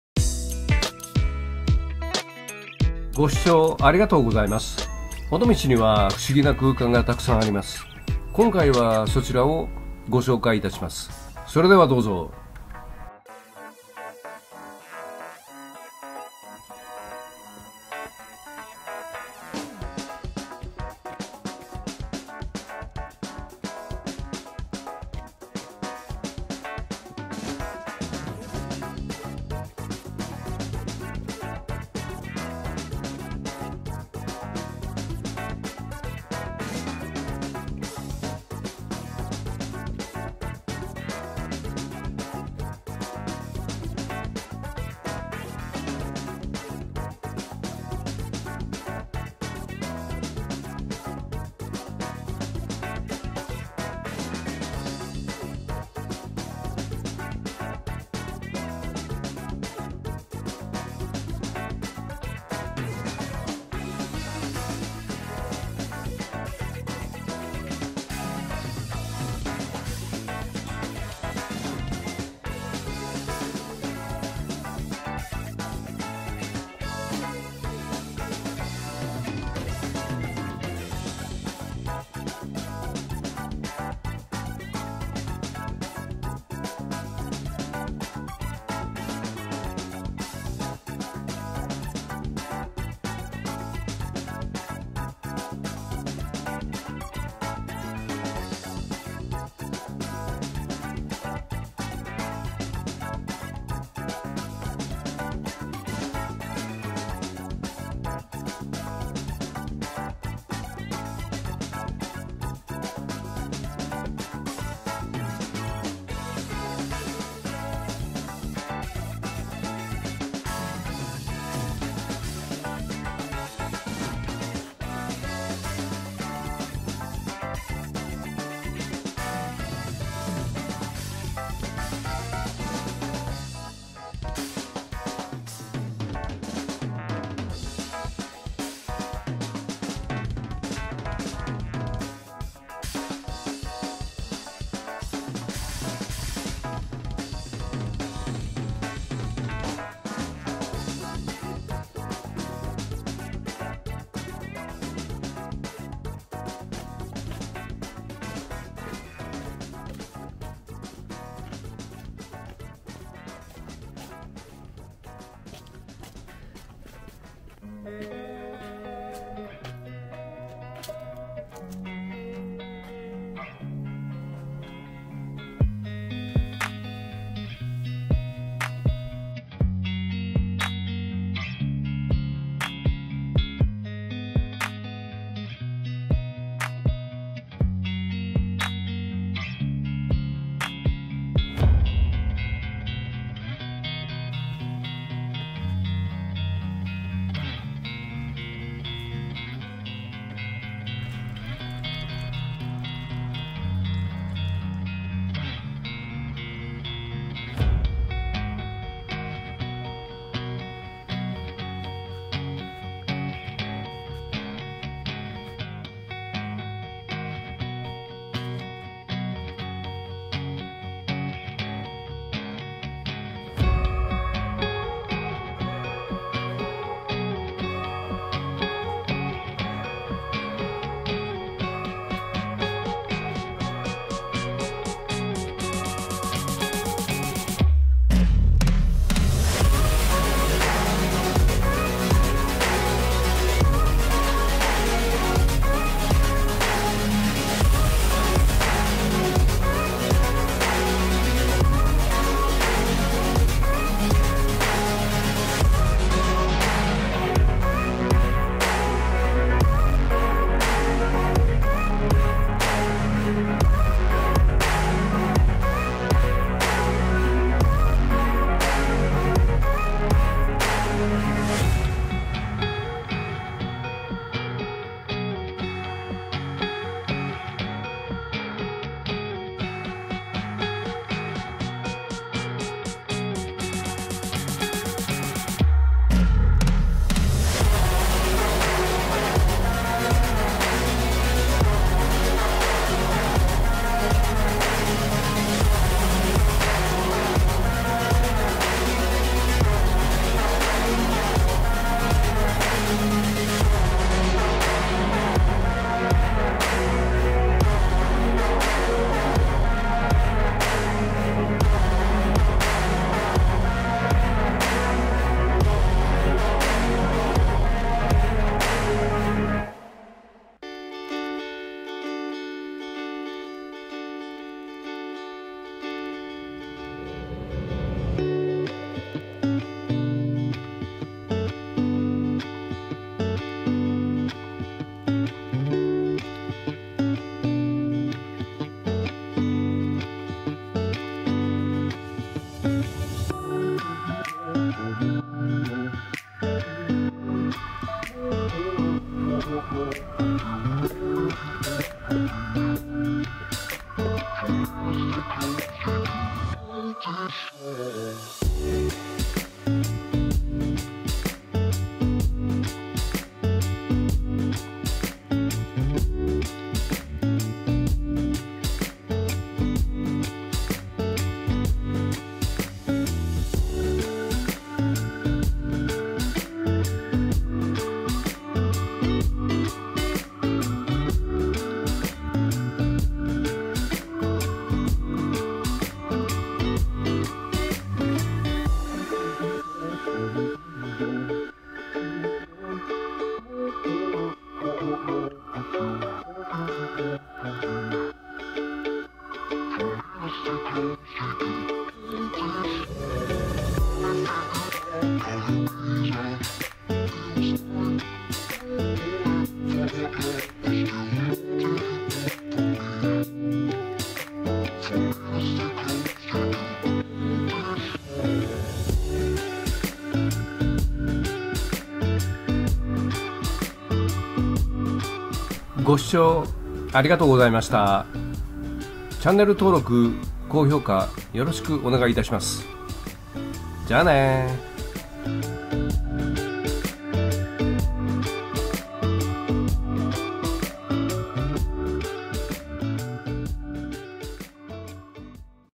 自称勝手に尾道観光大使「ワンダーゾーン編」
尾道には不思議ゾーンが沢山あります。
いくつかご紹介してます。